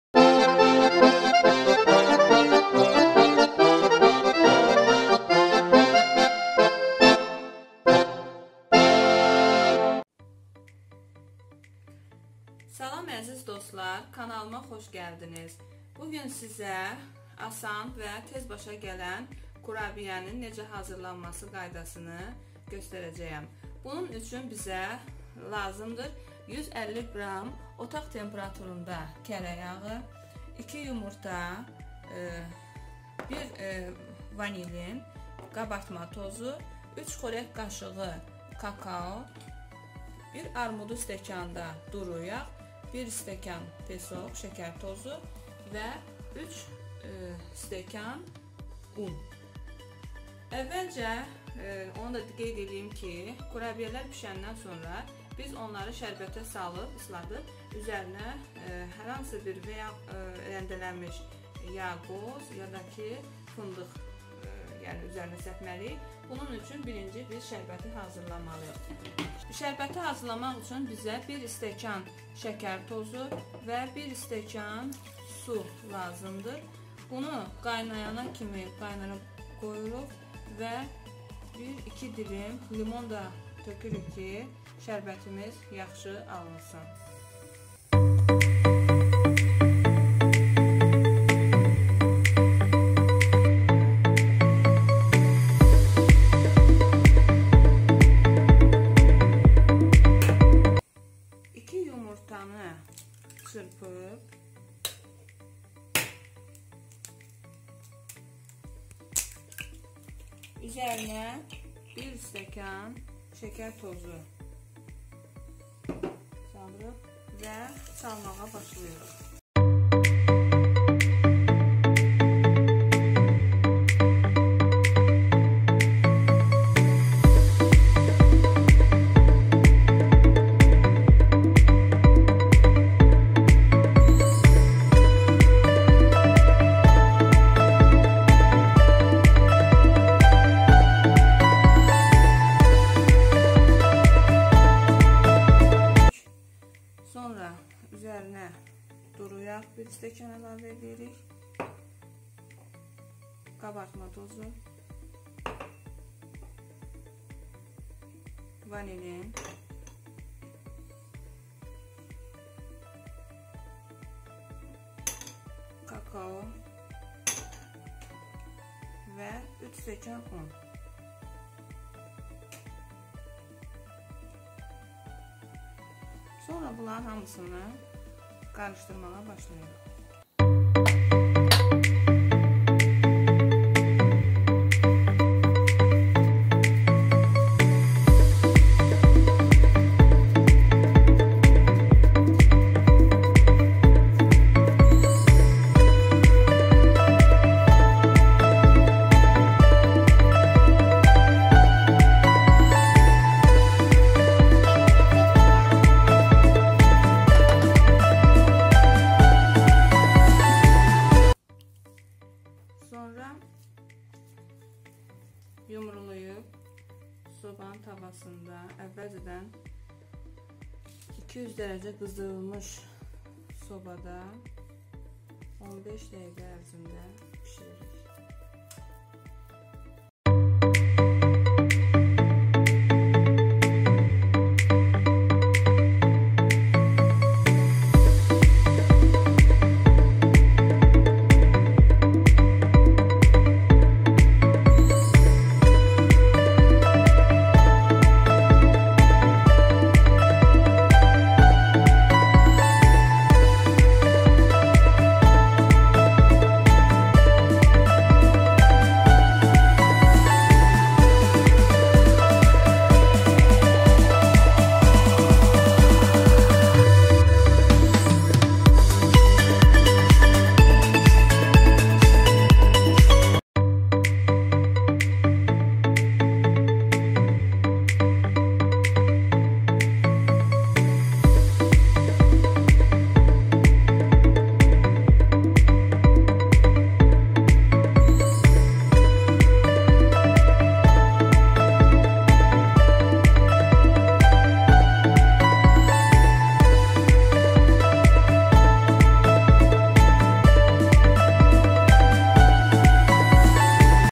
🔴Kurabiyə necə hazırlanır? Azərbaycan mətbəxi
Şirniyyatı sevənlər üçün pratik və ləzzət dolu bir reseptimiz var. Ən ləzzətli şirniyyatlardan biri olan kurabiyə. Bu resepti istifadə etmənizi məsləhət görərdik. İndidən Nuş Olsun!

♦️Resept:
150 qr yağ
2 yumurta
1 vanil
1 qabartma tozu
3 xörək qaşığı kakao
1 armudu stəkan duru yağ
1 stəkan şəkər tozu
3 stəkan un

♦️Digər videolarımız